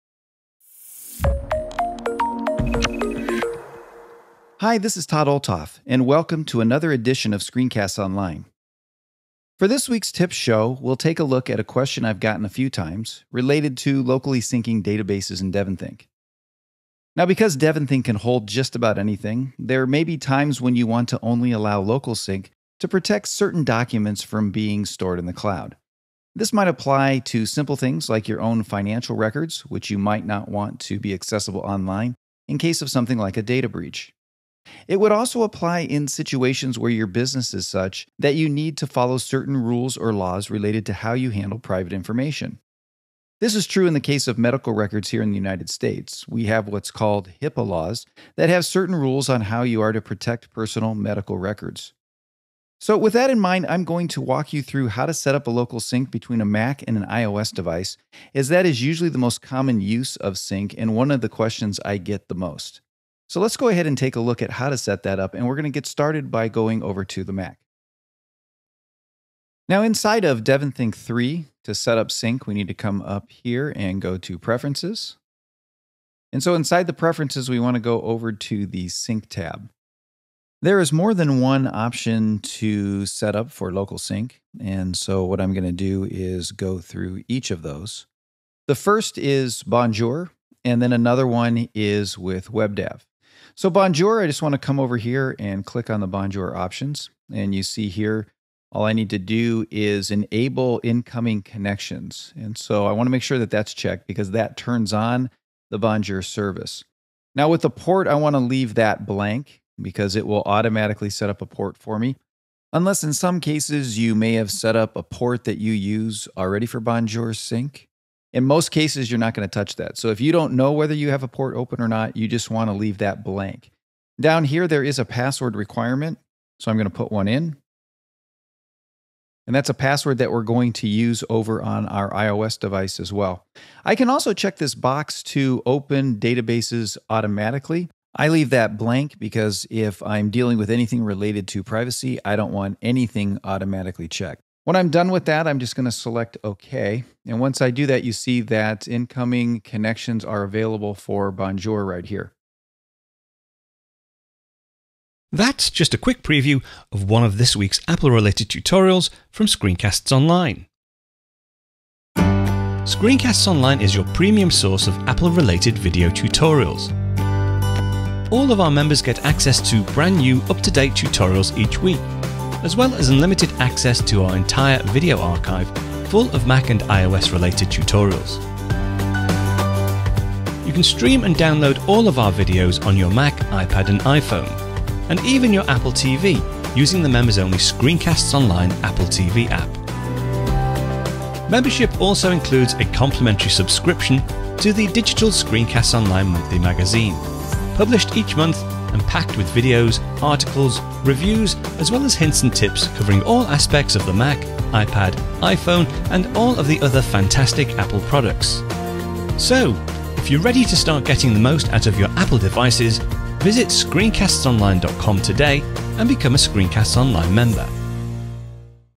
SCOM1114 - Use Local Sync with DEVONthink - Preview
This is a short preview of a full ScreenCastsOnline video tutorial. for a free 7-day membership. - There are times when you just don't want to use a cloud service to sync your important documents such as financial or business records. This is when local sync can help by syncing all your files on your server, while still having them accessible on your iOS devices on the go.

In this screencast, Todd walks you through setting up local sync on both Mac and iPad, using Bonjour and WebDAV.